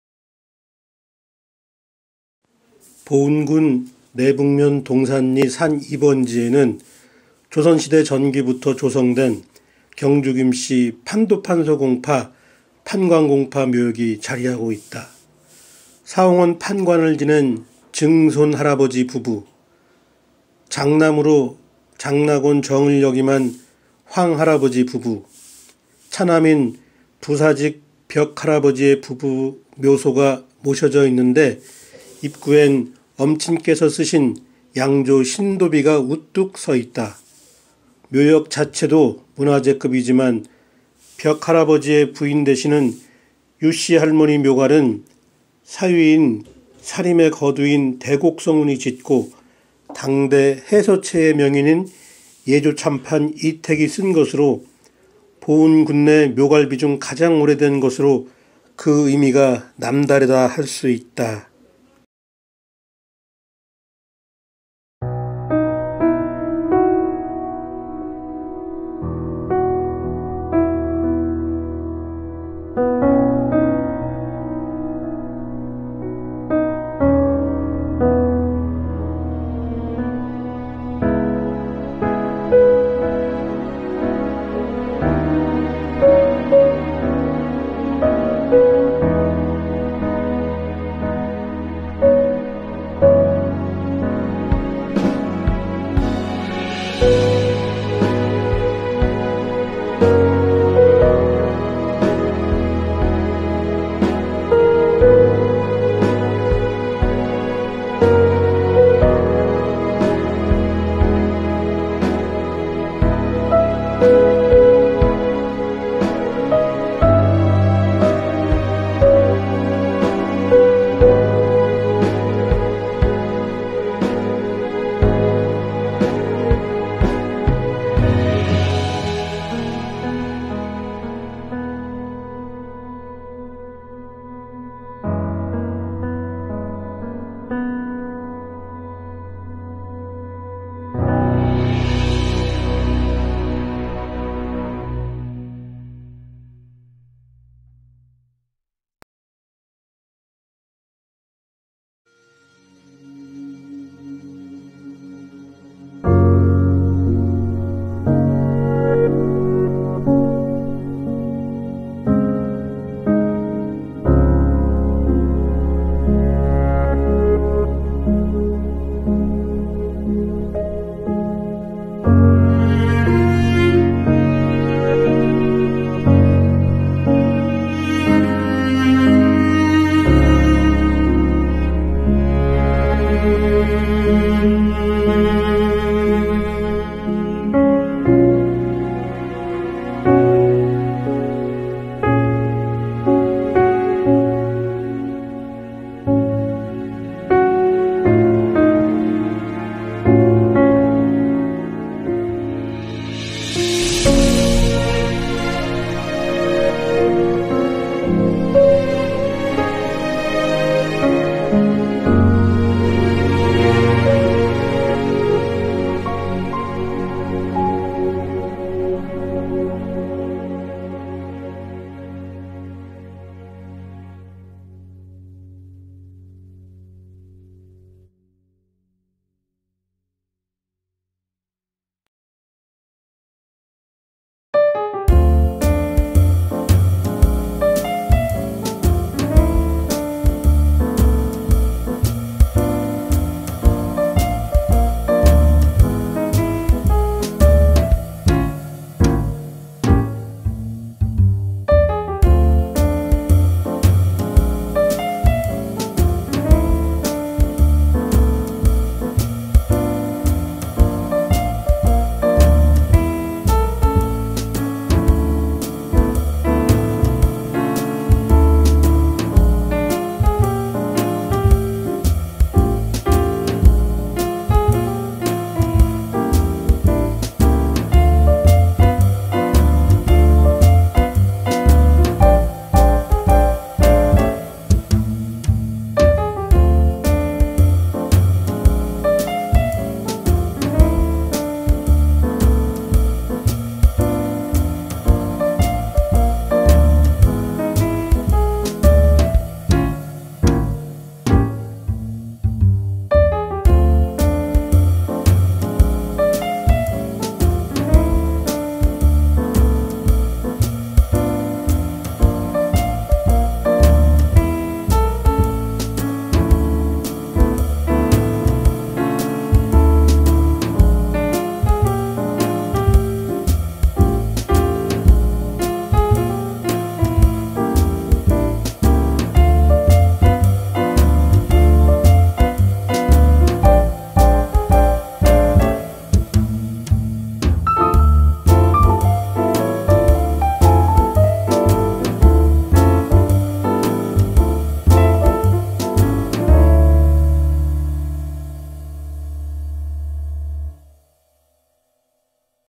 경주김씨 판도판서공파 판관공 묘역 내북면 동산리 사옹원 판관 김증손 부사직 김벽 장락원정 김황 숙부인문화유씨 대곡 성운 예조참판 이택 해서체 보은답사 보은관광 보은여행 김병서TV
보은군 내북면 동산리 산2번지에는 조선시대 전기부터 조성된 경주김씨 판도판서공 김장유의 자손 중 판관공파 묘역이 자리하고 있다.
사옹원 판관을 지낸 김증손 부부, 장남인 장락원 정을 역임한 김황 부부, 차남인 부사직 김벽 부부묘등이 모셔져 있는데 입구엔 엄친께서 쓰신 양조 신도비가 우뚝 서 있다.
묘역 자체도 문화재급이지만 벽 할아버지의 부인되시는 숙부인문화유씨 묘갈은 사위인 사림의 거두 대곡 성운이 짓고 당대 해서체의 명인인 예조참판 이택이 쓴 것으로 보은군내 묘갈비 중 가장 오래된 것으로 그 의미가 남다르다 할 수 있다.
영상 말미에 엄친이 쓴 "중직대부행사옹원판관경주김공휘증손신도비"와 "건공장군행충무위부사직증통정대부승정원좌승지겸연참찬관김공휘벽신도비" 내용과 유원대학교 호서문화연구소의 "역주 대곡집"에 수록된 대곡 성운 할아버지가 쓴 "류씨 묘갈명"을 첨부하였다"

中直大夫行司饔院判官 慶州金公諱曾孫神道碑
慶州金氏의 起源은 大輔公諱閼智로부터이니 大輔公은 西紀六十五年 新羅脫解尼師今九年 金城西쪽 始林나무가지에 걸린 金櫃에서 誕降하시였다. 王은 기뻐하며 姓을 金으로 下賜하여 이로부터 慶州金氏가 始作되었으니 그 根源이 深奧하다. 七世 味鄒가 助賁王의 女婿로 後嗣없이 昇遐하여 沾解王의 뒤를 이어 新羅十三代 王位에 卽位하여 王姓이 되고 敬順王이 高麗에 遜國할때까지 新羅五十六王 中 三十八王을 내어 三韓巨族이 되었다. 朝鮮世祖十四年 一四六八年戊子에 進士가되고 成宗朝에 中直大夫行司饔院判官 諱曾孫은 敬順王 第四子 侍中侍郞平章事輔國大安君諱殷說로부터 累傳하여 版圖判書諱將有로麗末政亂을 避하여 報恩에 隱遁하여 仍世居焉하니 그 五大孫이다. 高祖는 諱仲南이니 保勝中郞將兼監察糾正이고 曾祖諱乙湜은 判軍器寺事이며 祖는 諱滸로 平澤縣監贈都承旨요 考는 諱處庸으로 都染署丞贈兵曹叅判이고 妣는 贈貞夫人 上黨韓氏요 贈貞夫人 沃川黃氏는 宣略將軍諱憲의 女로 能通經史하고 敎子義方하시니 公은 卽己卯名賢 文簡公 冲菴의 伯父이시다. 配位는 淑人全州李氏副護軍 諱 處東의 女이시다. 四男을 生하시니 長黃은 燕山十年에 別試文科하여 承文院叅校贈掌樂院正이고 次碧은 副司直贈左承旨요 次赤은 成均生員이고 次白은 无后이다. 叅校系子는 天宙니 禮賓寺別坐요 司直男 天富는 進士 金堤郡守贈掌樂院正이요 天宇는 中宗三十三年 別試文科로 弘文館典翰贈副提學이고 天宙는 出系하고 進士公赤의 男은 天日이며 女는 永山金繼勳과 昌寧 成運은 號大谷으로 司宰監正贈左承旨로 冲菴과 함께 享象賢書院이다. 別坐男可珍은 无后이고 女는 全義李淮壽로 水使요 許儐은 生員이다. 郡守男 可晦는 長仕郞이고 可恒은 虞候며 可明이요 可權은 牧使로 壬辰倭亂에 殉節하여 无后요 可幾는 魯城縣監贈左承旨이고 可根과 可禎이며 女는 金權과 吳元起와 吳汝舟와 成聞德과 李端과 金農은 司評이다. 典翰男 可賓은 司憲府監察贈兵曹叅判이요 可宗은 宣敎郞이다. 可晦男 峯壽는 贈軍資監正이고 希哲은 壬辰亂殉節로 享後栗祠하고 希傑이며 女는 李茂와 李億慶守門將과 吳甲과 安夢成이고 可恒南 崑壽는 東明尉 贈吏曹判書요 嵋壽와 岑壽요 可明男은 峻壽와 崙壽와 垈壽요 可幾男 德民은 僉知中樞府事贈戶曹叅判으로 夫人贈貞夫人 高靈申氏는 丁酉再亂에 殉節하여 命旌되었으며 可根男은 有男福男善男이고 可禎男은 岐壽이다. 可賓男은 汴으로 贈純忠補祚功臣戶曹判書月川君이고 女는 朴繼熙와 申光弼과 崔五鳳이고 可宗男滌은 贈童蒙敎官이고 澣과 涉은 无后이고 女는 柳璋은 同知요 韓瑷과 崔挺豪 正言이다. 汴男 元亮은 逸司憲府持平이니 策靖社勳하여 封月城君하고 贈戶曹判書諡剛愍으로 命不祧廟하였다. 公의 生年은 失傳이고 忌日은 五月十日이라 世宗朝 一四四十年初에 誕生하시어 燕山君때인 一五00年前後에 卒하신 것으로 推定할뿐이니 鳴呼哀哉라 遠孫은 다만不勝飮泣하노라
西紀二千十一年 歲在 辛卯 五月 端午節
第十七代孫 報恩文化院長 建植 謹撰
判官公 承旨公 兩世祖 神道碑 建立 推進委員會 謹竪

建功將軍 忠武衛副司直 贈通政大夫承政院左承旨兼筵叅贊官 慶州金公諱碧神道碑
公의 諱는 碧이고 字는 孟仁이요 姓은 金으로 慶州가 貫이다 慶州金氏는 新羅의 宗姓으로 敬順王 第 四 王子 輔國大安君 휘 殷說이 累傳하여 諱 將有하니 官版圖判書로 麗末政亂을 避하여 報恩에 隱居하여 代代로 살게되었으니 版圖判書公派의 派祖이시다. 判書公이 生 諱 仲南하니 保塍中郞將兼監察糾正으로 公의 五代祖이다.
高祖는 諱 乙湜으로 判軍器寺事이고 曾祖 諱 滸는 平澤縣監贈都承旨요 祖는 諱 處庸으로 都染署丞贈兵曹叅判이고 考는 諱 曾孫으로 司饔院判官이시고 妣는 淑人全州李氏로 副護軍 諱 處東의 女이다.
公은 成宗 四年인 一四七三年 癸巳에 鐘谷本第에서 判官公의 四南中 次男으로 出生하셨으니 伯兄은 諱 黃으로 登第하여 官至承文院叅校 贈掌樂院正이고 第는 成均生員赤과 白이다. 公은 幼時부터 聰明하여 才能이 뛰어났으며 愼重하고 寡默함이 成人같아 祖母貞夫人黃氏가 鐘愛하였다. 貞夫人은 沃川望族으로 婦德이 높고 能通經史傳하여 子孫을 嚴格하게 敎訓하니 由是로 公은 德器가 夙成하여 少年時에 名聲을 얻었으며 長成하여 氣像이 높고 學問이 豊富하여 文士들이 推重하였으며 從弟인 文簡公 冲菴이 敬愛하여 常時從遊하였다. 公의 資稟은 正直方正하고 剛健하여 惡을 멀리하고 潔己自重하니 鄕隣의 老少가 모두 敬畏하여 말하기를 不義는 金大人같이 물리쳐야한다 하였다. 또한 子弟를 敎育할 때 學問은 반드시 正道로 배우되 擧業을 爲해 奪志하지 않도록 恒常警戒하가 하였으니 사람됨이 이와 같이 篤實하고 善良하였다. 蔭職으로 忠武衛副司直이 除授되었으나 나는 大夫의 後裔로 이는 늙어 田園에 있음과 같도다하고 仕進할 것을 勸誘받았으나 마침내 不仕하고 뒤에 子孫의 貴로 通政大夫承政院左承旨兼經筵叅贊官에 贈職되었다. 嘉靖十二年인 中宗二十八年癸巳六月十六日 以疾로 考終命하니 年六十一歲였다. 期年十月初四日 葬于靑山屬縣酒城今內北東山蘆山之原先塋之側하였다. 配位는 贈淑夫人文化柳氏로 修義副尉依江의 女로 有淑德하여 婦德이 높았다. 生三男二女로 長男은 天富로 以進士로 金堤郡守 贈掌樂院正이고 次天宇는 登第後 官至弘文館典翰하고 贈副提學이며 次天宙는 禮賓寺別坐로 出系伯父叅校公이며 女는 長適永山金繼勳副護軍이요 次는 昌寧成運으로 號大谷이며 司宰監正贈左承旨로 亨本郡象賢書院이다. 郡守公이 生七男六女로 長男은 可晦로 長仕郞이고 次可恒은 虞候며 可明이요 可權은 壬辰亂에 殉節하여 无后요 可幾는 魯城縣監贈左承旨이고 可根과 可禎이며 女婿는 金權과 吳元起와 吳汝舟와 成聞德과 李端과 金農은 司評이다. 典翰公이 二男으로 長可賓은 司憲府監察贈兵曹叅判이고 次可宗은 宣敎郞이다. 可晦男峯壽는 贈軍資監正이고 希哲은 壬辰亂殉節로 享後栗祠요 希傑이며 女婿는 李茂와 守門將李億慶과 吳甲과 安夢成이고 可恒南崑壽는 東明尉贈吏曹判書요 嵋壽와 岑壽요 可明男은 峻壽와 崙壽와 垈壽이고 可幾男德民은 僉知中樞府事贈戶曹叅判이고 夫人贈貞夫人高靈申氏는 丁酉再亂에 殉節하여 命旌되었으며 可根男은 有男과 福男과 善男이고 可禎男은 岐壽이다. 可賓男汴은 贈純忠補祚功臣戶曹判書月川君이고 女婿는 朴繼熙와 申光弼과 崔五鳳이고 可宗男滌은 贈童蒙敎官이고 澣과 涉은 无后이고 女婿는 同知柳璋과 韓瑷과 正言崔挺豪이다. 汴男元亮은 逸司憲府持平으로 策靖社勳하여 封月城君으로 贈戶曹判書諡剛愍으로 命不祧廟되었으며 以下는 蕃衍하여 不錄한다. 銘하니 慶州之金이 隱居鍾山하여 子孫이 繩繩하고 搢紳相傳은 判官積德과 承旨積善之功이라 蘆山地氣와 兩祖蔭德이 相承하니 後昆榮達이 無窮하리라.
西紀二千十一年 歲在 辛卯 五月 端午節     第十六代孫 報恩鄕校原任典校 建植 謹撰